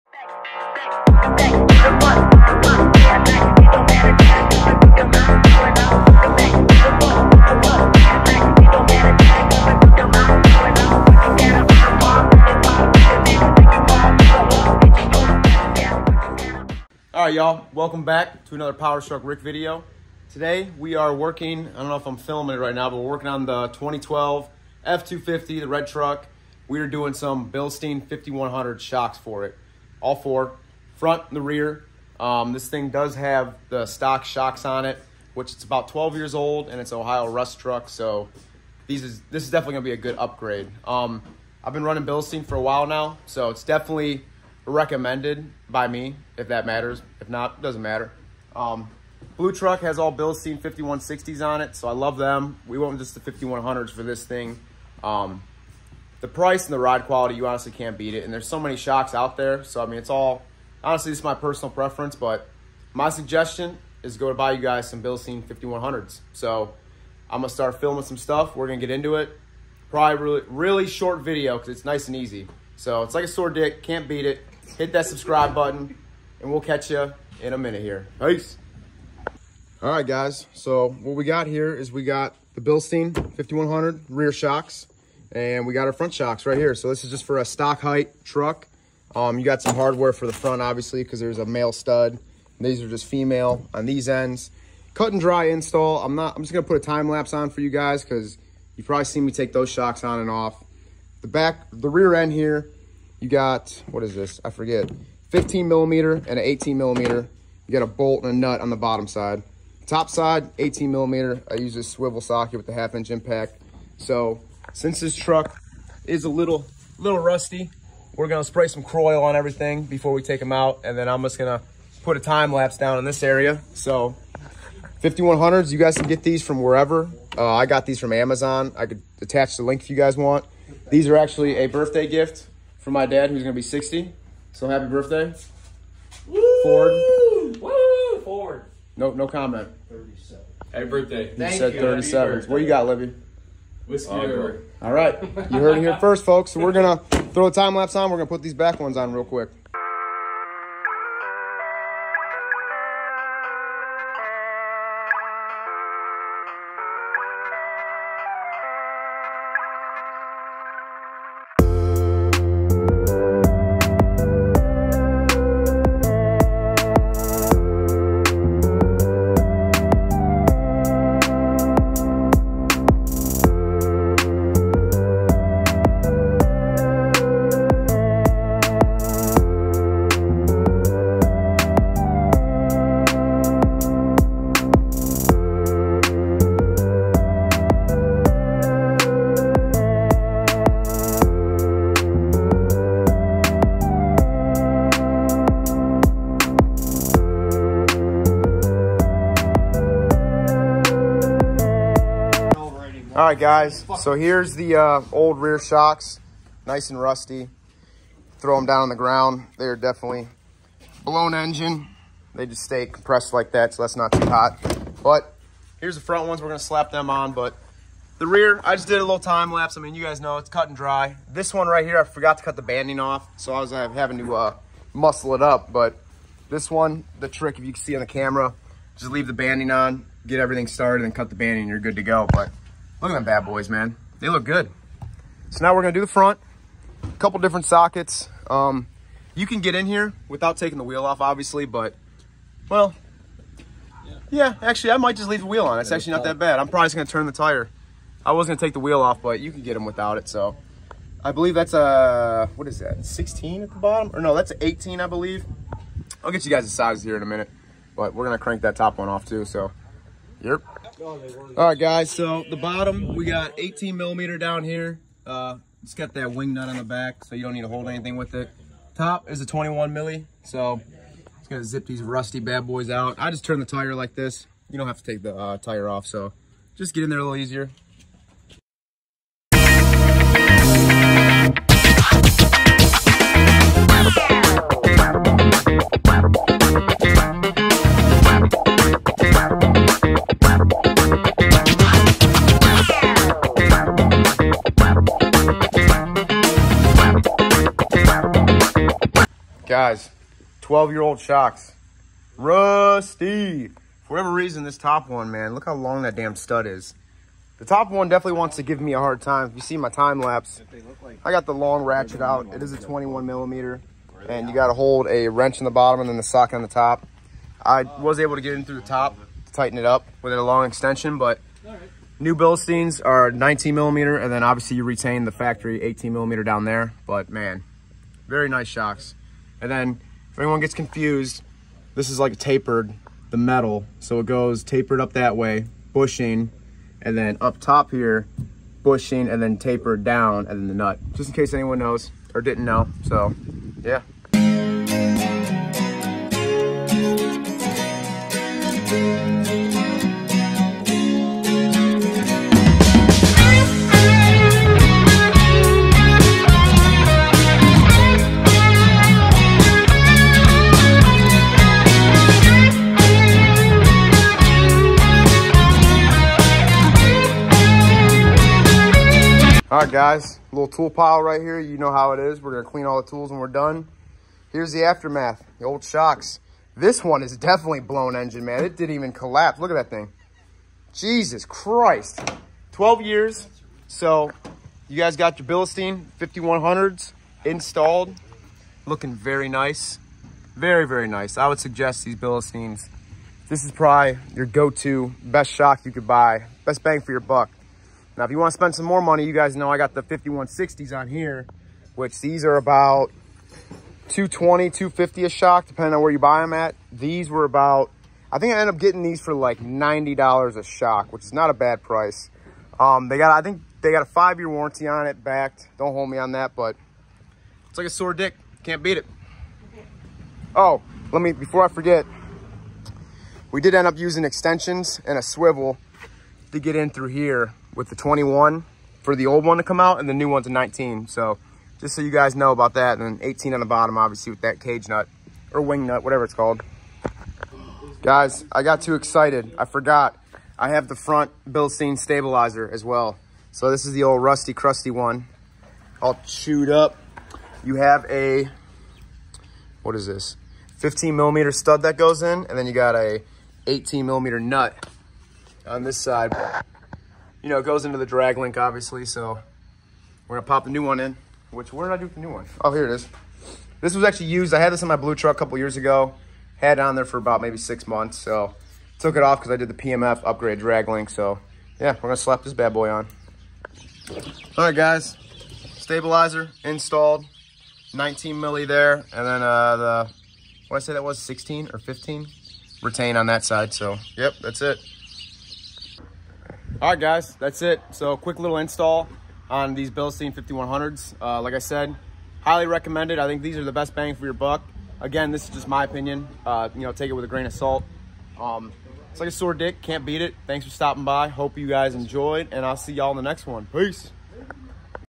The BEST SHOCKS to run on your FORD F-250 SUPER DUTY!? What is your thoughts?
What’s up y’all thanks for stopping by!! You already know it’s like a sore dick, can’t beat it!! In this video I install some bilstein 5100 shocks and stabilizer!! In my opinion these are the best bang for your buck! What do you think?!